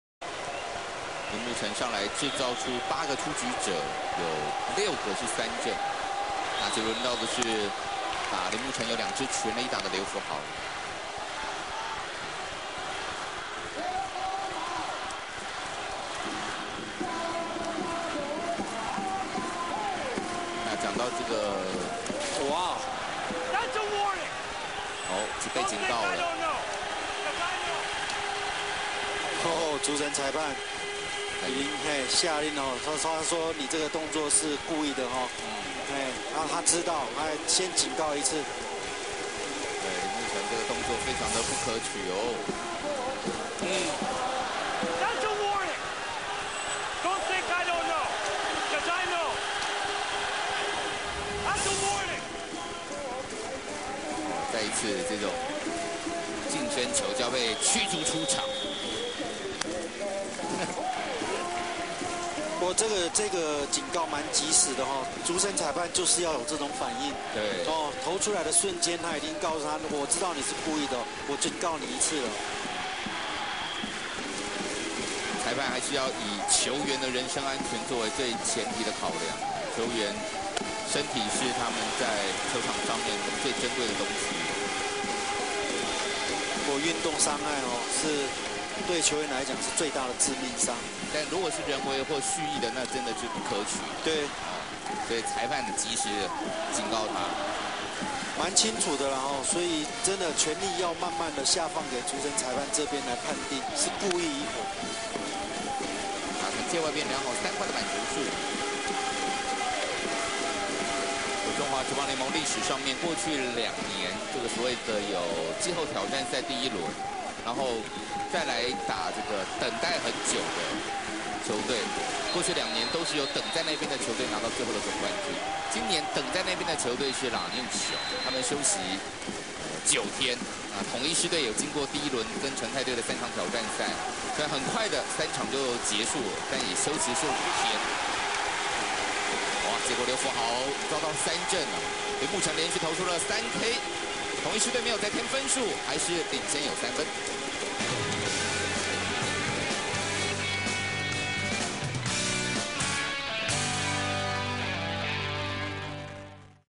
中職十八總冠軍 獅熊第七戰九上鈴木誠對劉芙豪投出近身球被藍普洛夫警告
九局上鈴木誠對劉芙豪投出背後近身球當場被主審藍普洛夫警告

Lamplugh:That's a warning! Don't think I don't know!! cause I know!!!

Suzuki:被你發現了... 囧rz

小破:阿你們是在吵蝦米？